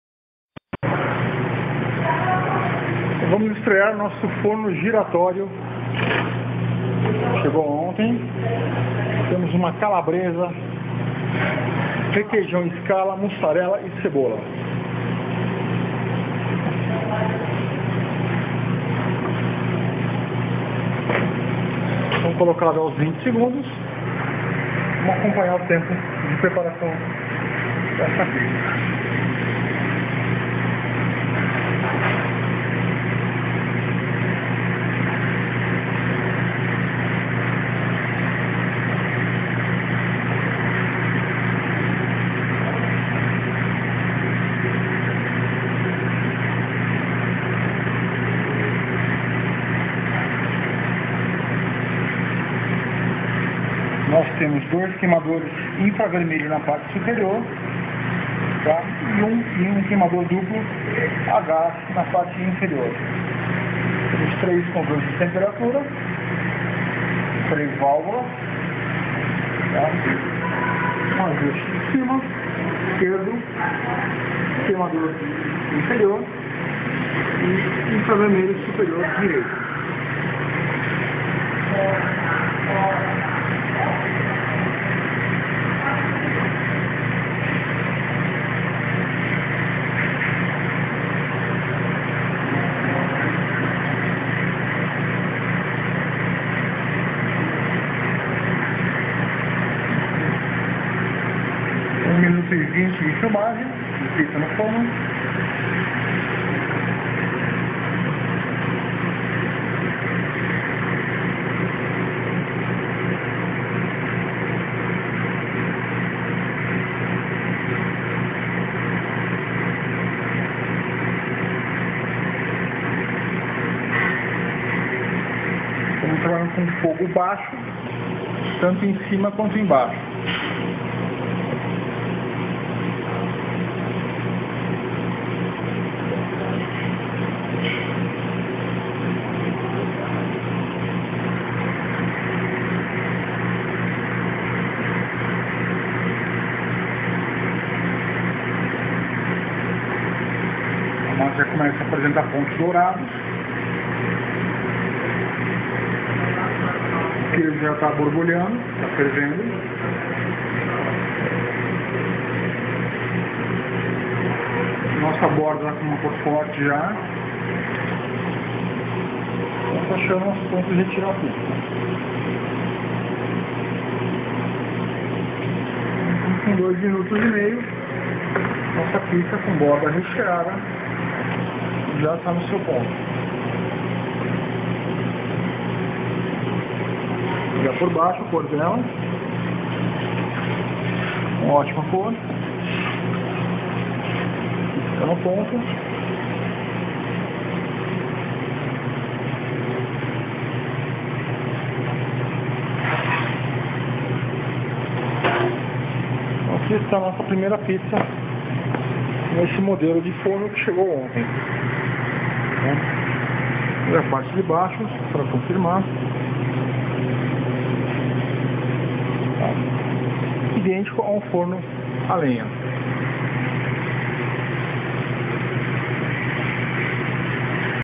Forno Giratório Caffé Coretto - Cheff William
Forno de Pizza Giratório, rápido e prático. Pizza identica a forno a lenha